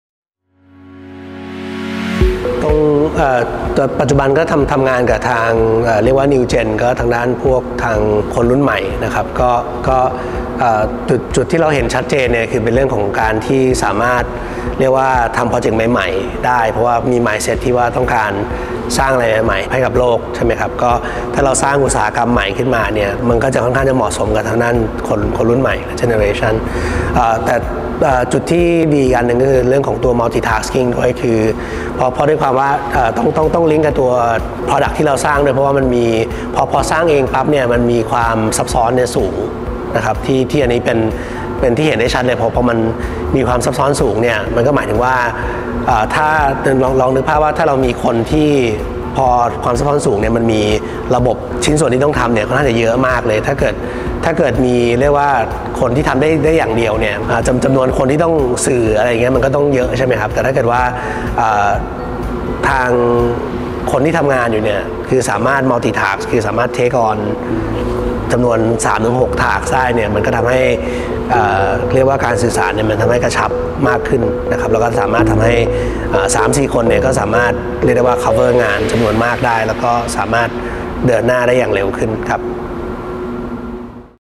Why does mu Space see the potential in New Gens? | James Yenbamroong (Founder, CEO, and CTO)
The aerospace industry can still be considered a fairly new industry that requires high-end technology therefore having experienced members is important. However, the reason mu Space focuses on hiring New Gens is because we believe that they have enough quality and skills to push the aerospace industry further. James Yenbamroong, Founder, CEO, and CTO of mu Space gives his thoughts on this subject in the following video:

“อวกาศ” เป็นอีกหนึ่งอุตสาหกรรมที่ถือได้ว่า ค่อนข้างใหม่ และต้องใช้เทคโนโลยีขั้นสูง ดังนั้น การมีผู้เชี่ยวชาญในด้านต่าง ๆ เข้ามาร่วมทีมจึงอาจเป็นเรื่องที่จำเป็นไม่น้อย แต่เพราะเหตุใด mu Space ถึงเล็งเห็นว่า คนรุ่นใหม่นั้นมีศักยภาพเพียงพอที่จะสามารถเข้ามาร่วมขับเคลื่อน และผลักดันวงการธุรกิจอวกาศได้ เรามาฟังความคิดเห็นในเรื่องนี้จาก คุณเจมส์ วรายุทธ เย็นบำรุง Founder, CEO และ CTO ของ mu Space ได้ในวิดีโอนี้ไปพร้อมกัน!